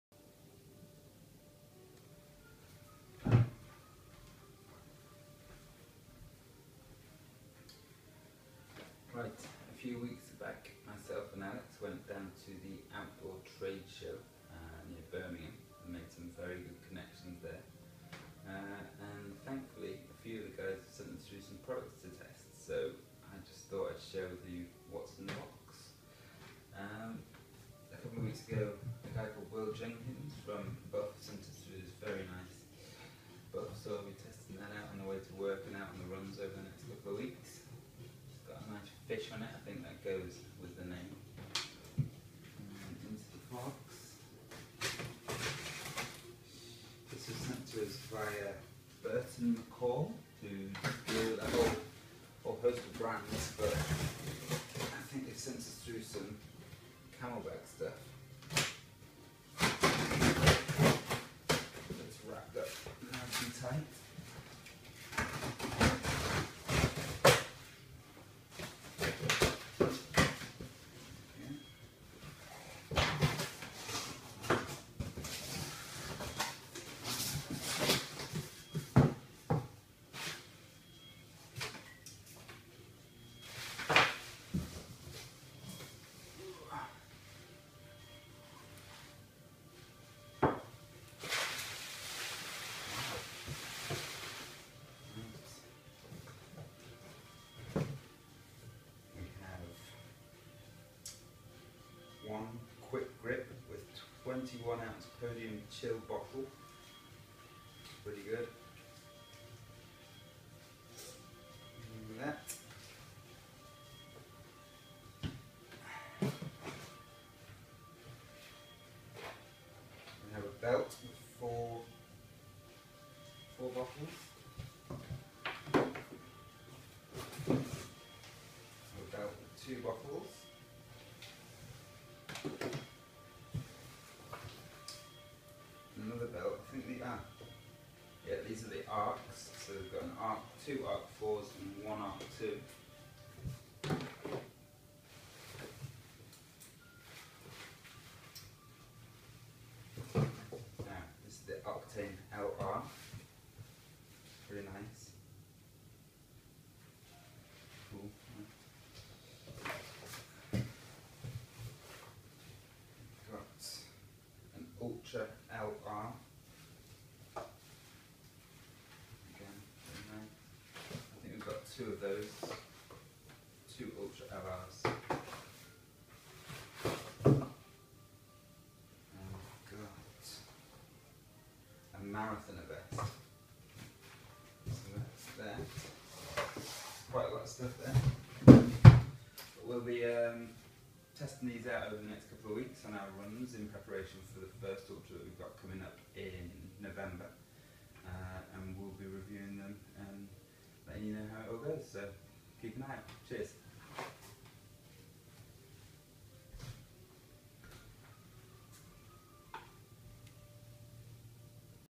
Camelbak and Buff Gear
Products arrive. What's in the box and look out for product reviews over the next couple of weeks! All reviews will be open & honest